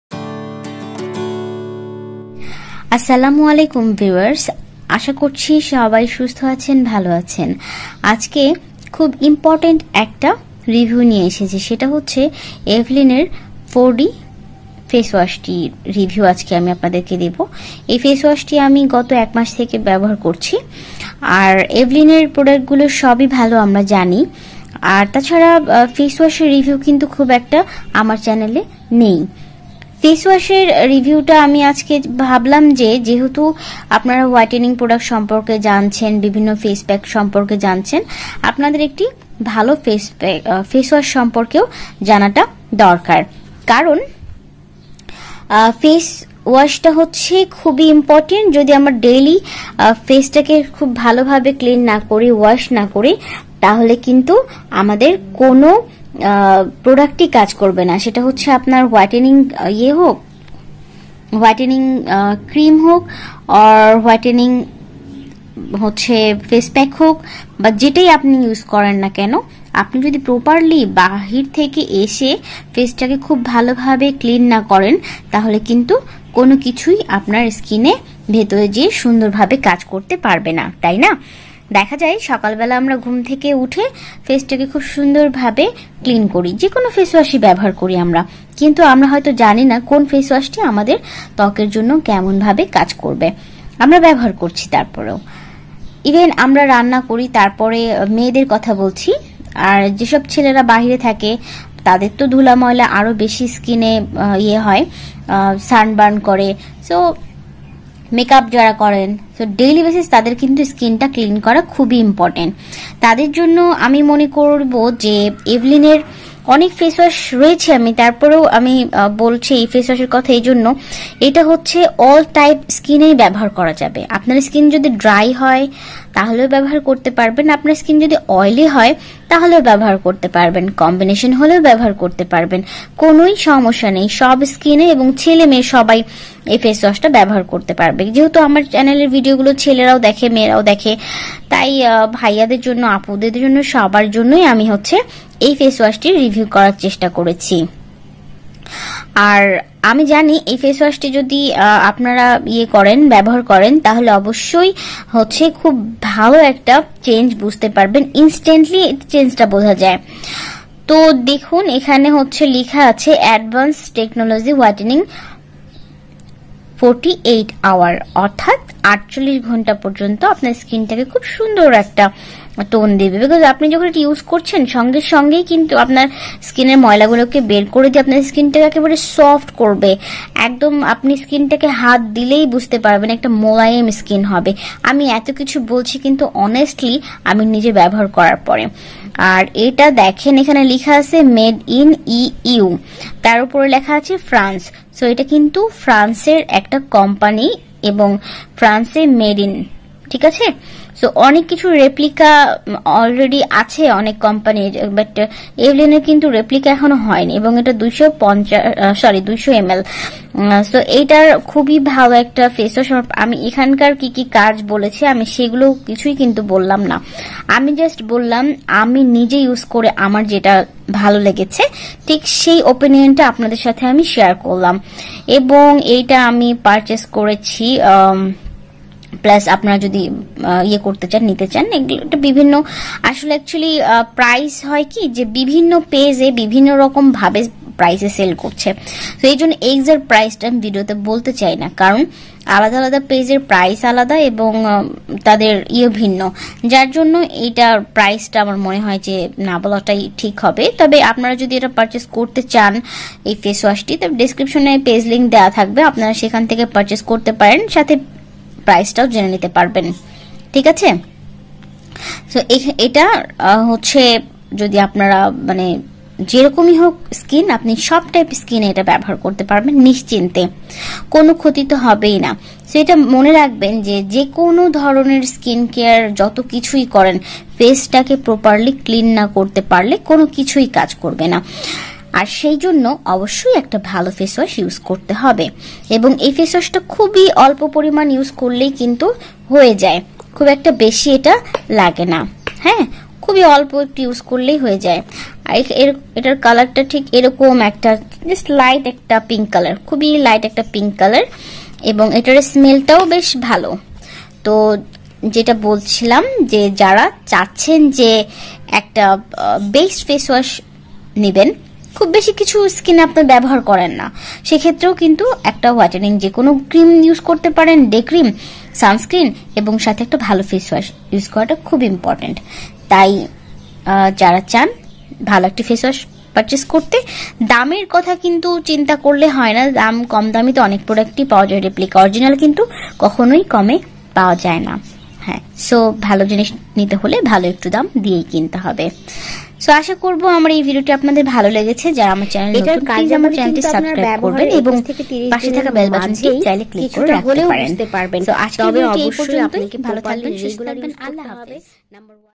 EVELINE 4D whitening facial gel bangla review।। এই ফেসওয়াস টি আপনার ত্বকের জন্য কতটা কার্যকরী!
অর্ডার করতে যোগাযোগ করুন এই লিঙ্কে